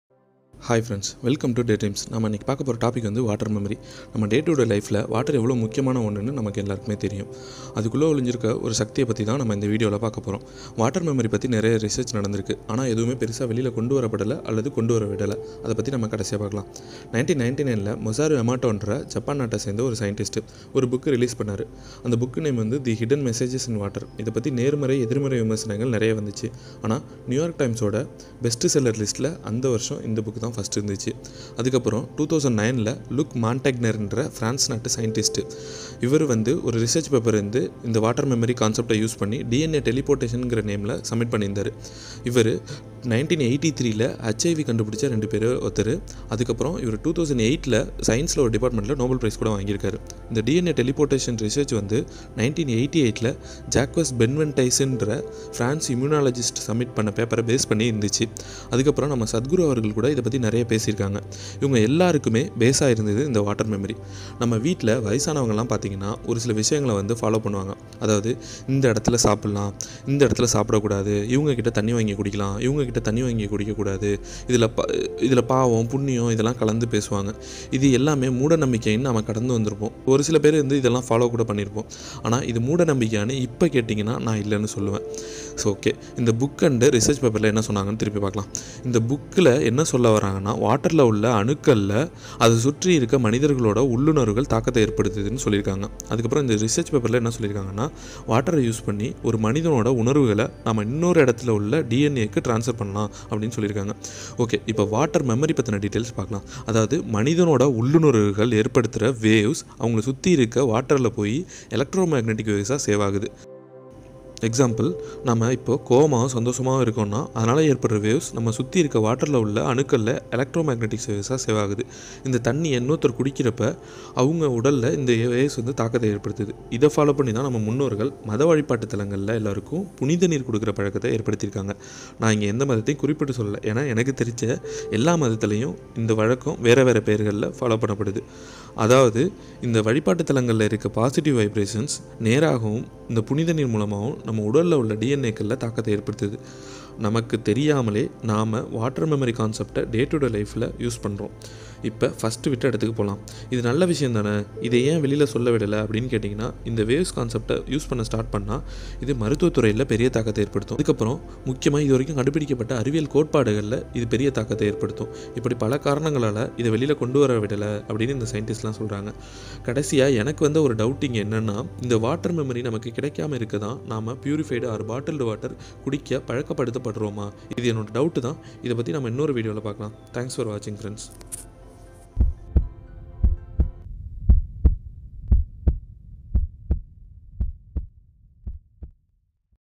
Secret of Water in tamil | Water Memory| The Power of Water |science | belives
Water memory theory of various scientist compared with daytoday practices